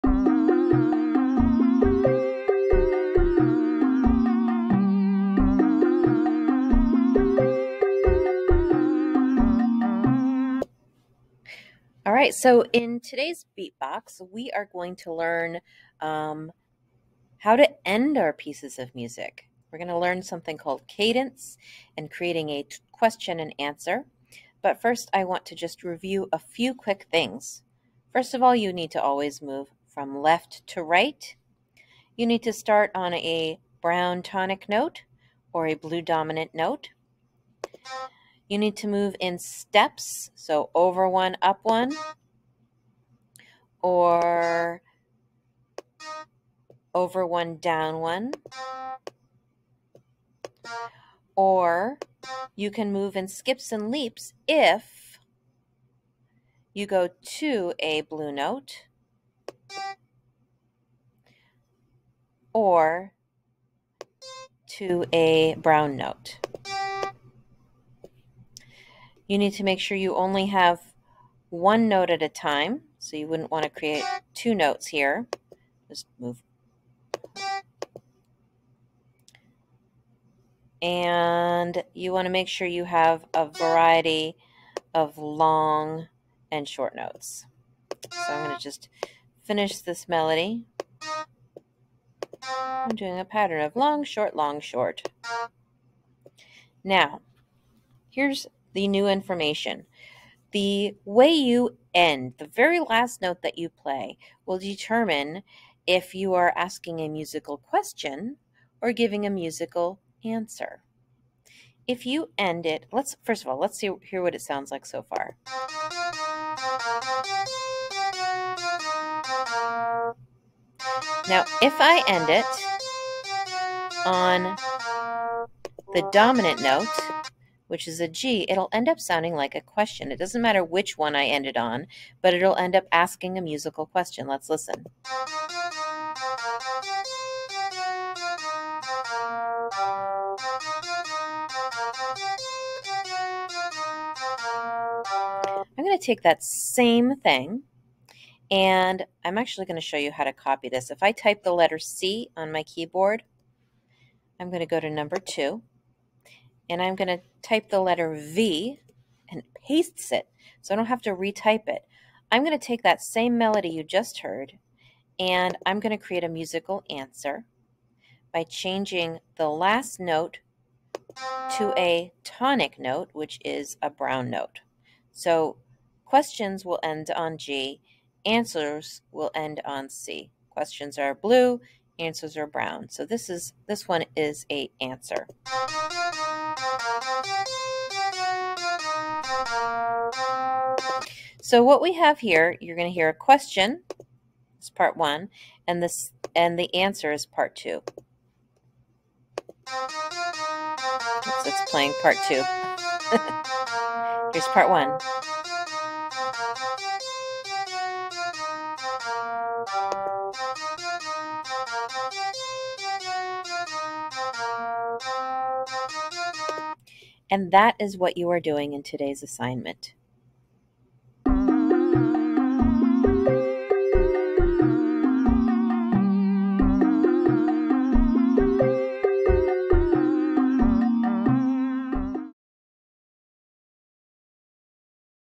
Beepbox Melodies part 3: Cadence question and answer
This video is part 3 in a series of videos that teach composition in a program called "Beepbox."

A cadence is just the way you end a musical phrase. They can sound more like a question (technical term is a "half cadence") or an answer (technical term is a "full cadence.) You can do this by simply changing the last note of your song. If you end on tonic (in which is a brown C in this song) it will be sound like a question, and if you end on a dominant note (in this song its a blue G).

Here is a beepbox starter to create your own melodies.